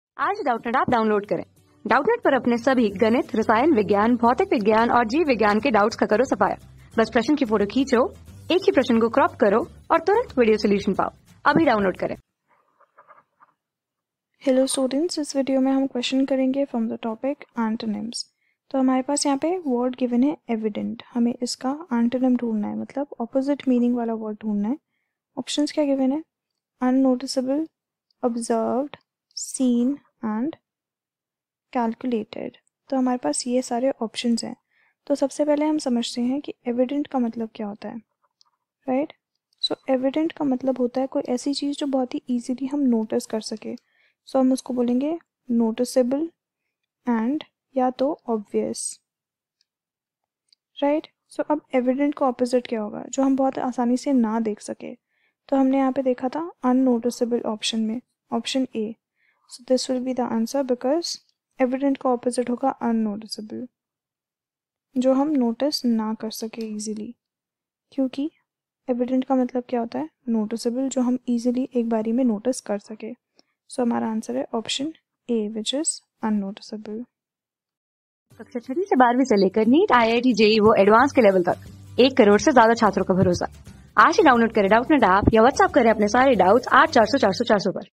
In the following items, which of the given word is opposite in meaning of the words provided. EV...
In the following items, which of the given word is opposite in meaning of the words provided. EVIDENT
Class: 12
Subject: ENGLISH
Chapter: ANTONYMS
Board:NDA

👉 Have Any Query? Ask Us.